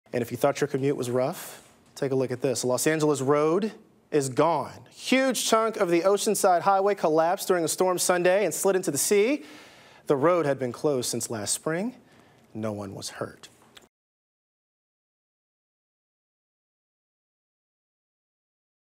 Los Angeles road collapses into sea
A huge chunk of the Oceanside Highway in Los Angeles collapsed during a storm and slid into the sea. Terrell Brown reports.